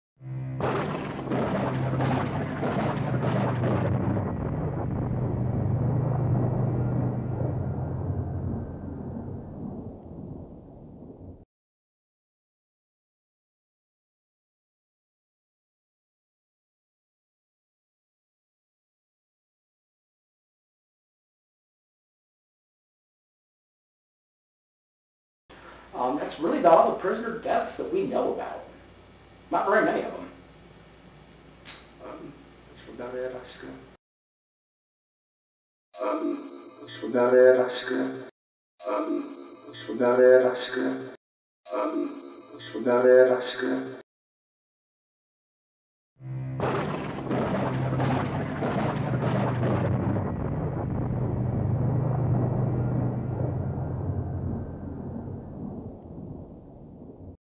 Squirrel Cage Jail Foreign Language
This EVP was captured at the Squirrel Cage Jail in Council Bluffs, IA.  The EVP clip is of a different unrecognizable language.
For more information on the Squirrel Cage Jail or to see more evidence from PRISM (Paranormal Research and Investigative Studies Midwest) visit our